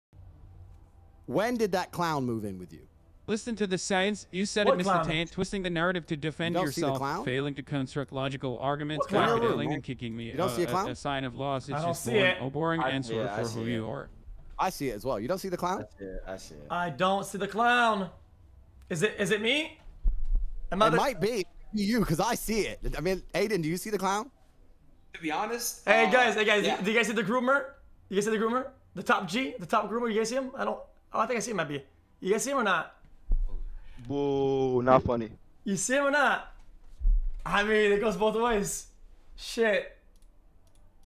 XQC Calls Out Andrew Tate For Being A Groomer!
Clip of: XQC
Clip By: NoobAlike